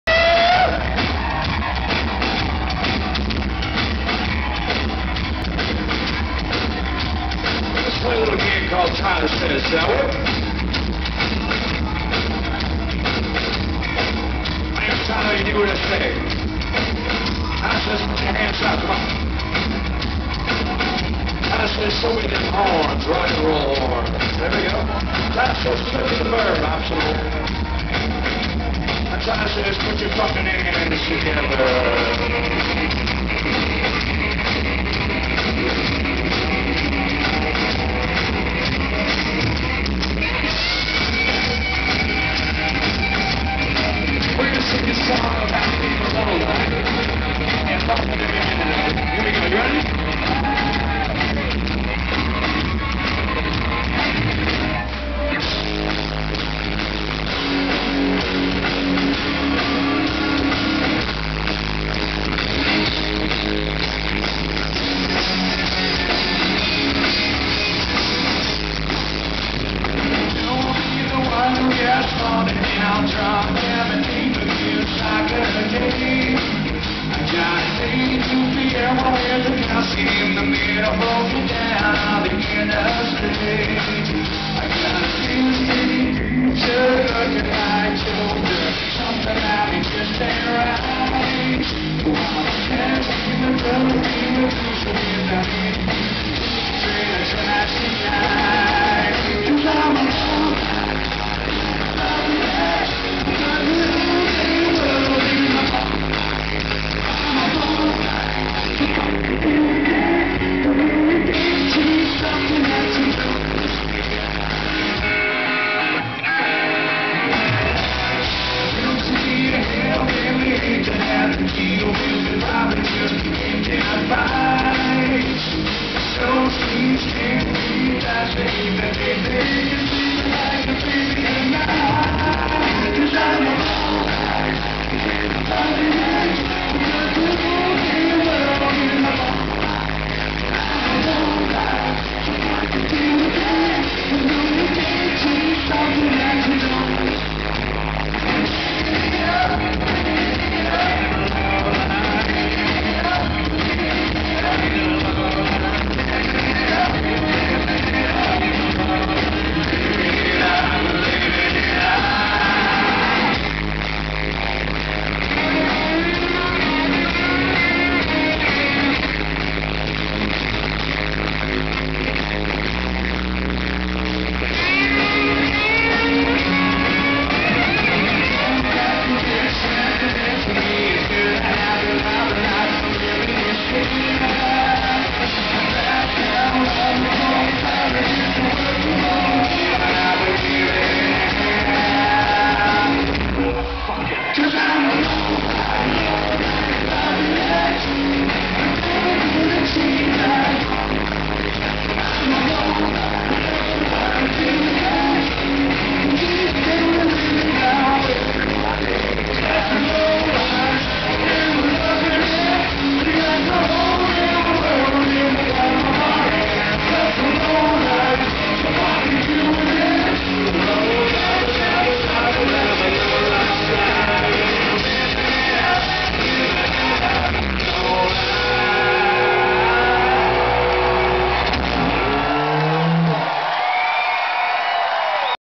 Theory of a deadman live
Theory of a deadman live in Buffalo NY 4/28/12